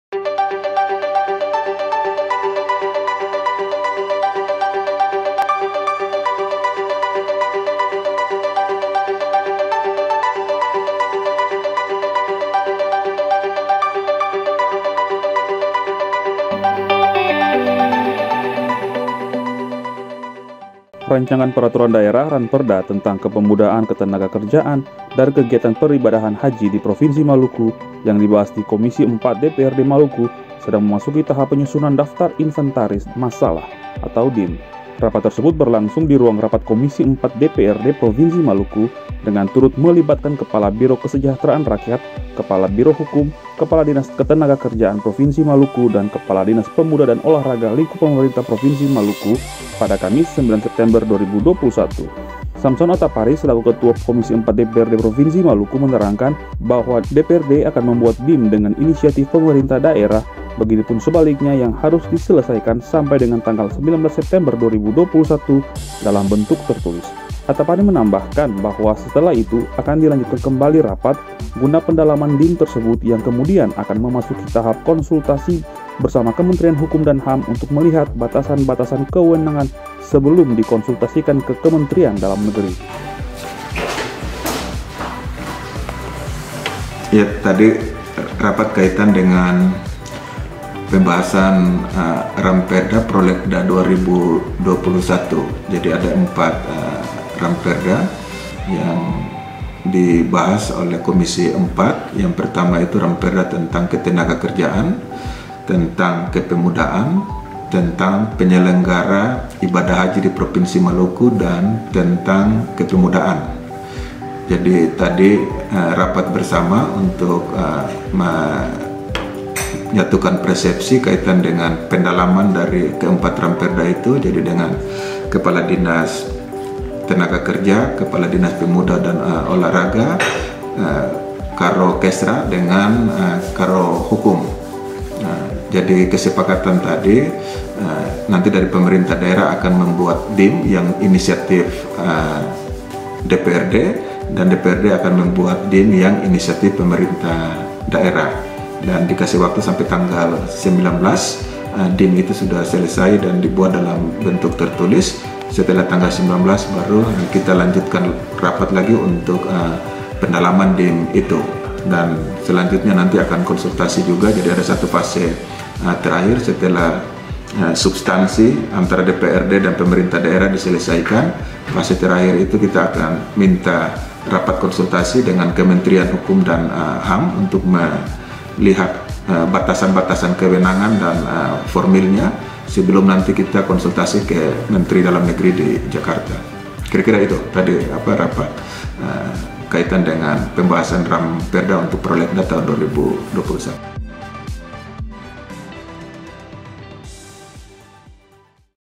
DPRD : RANPERDA PROLEGDA 2021 MEMASUKI TAHAP PENYUSUNAN DIM
RANCANGAN PERATURAN DAERAH (RANPERDA) TENTANG KEPEMUDAAN, KETENAGAKERJAAN, DAN KEGIATAN PERIBADAHAN HAJI DI PROVINSI MALUKU YANG DIBAHAS KOMISI 4 DPRD MALUKU, SEDANG MEMASUKI TAHAP PENYUSUNAN DAFTAR INVENTARIS MASALAH (DIM).

RAPAT TERSEBUT BERLANGSUNG DI RUANG RAPAT KOMISI 4 DPRD PROVINSI MALUKU DENGAN TURUT MELIBATKAN, KEPALA BIRO KESEJAHTERAAN RAKYAT, KEPALA BIRO HUKUM, KEPALA DINAS KETENAGA KERJAAN PROVINSI MALUKU, DAN KEPALA DINAS PEMUDA DAN OLAHRAGA LINGKUP PEMERINTAH PROVINSI MALUKU PADA KAMIS, 9 SEPTEMBER 2021.

SAMSON ATAPARY SELAKU KETUA KOMISI 4 DPRD PROVINSI MALUKU MENERANGKAN BAHWA DARI HASIL RAPAT KOMISI 4 DPRD MALUKU BERSAMA PEMERINTAH DAERAH IALAH DPRD AKAN MEMBUAT DIM DENGAN INISIATIF PEMERINTAH DAERAH BEGITUPUN SEBALIKNYA YANG HARUS DISELESAIKAN SAMPAI DENGAN TANGGAL 19 SEPTEMBER 2021 DALAM BENTUK TERTULIS.

ATAPARY MENAMBAHKAN BAHWA SETELAH ITU AKAN DILANJUTKAN KEMBALI RAPAT, GUNA PENDALAMAN DIM TERSEBUT  YANG KEMUDIAN AKAN MEMASUKI TAHAP KONSULTASI BERSAMA KEMENTERIAN HUKUM DAN HAM UNTUK MELIHAT BATASAN-BATASAN KEWENANGAN SEBELUM DI KONSULTASIKAN KE KEMENTERIAN DALAM NEGERI.